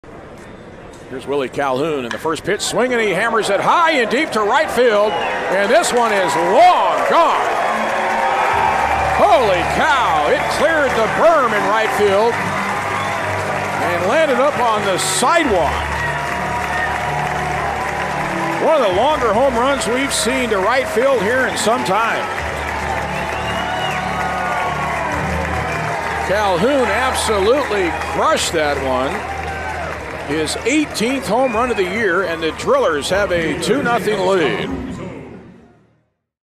Willie Calhoun Hits a Bomb
Los Angeles Dodgers prospect Willie Calhoun is on fire. Calhoun's 18th home run of the season was nearly blasted out of the park. ‪#‎Dodgers‬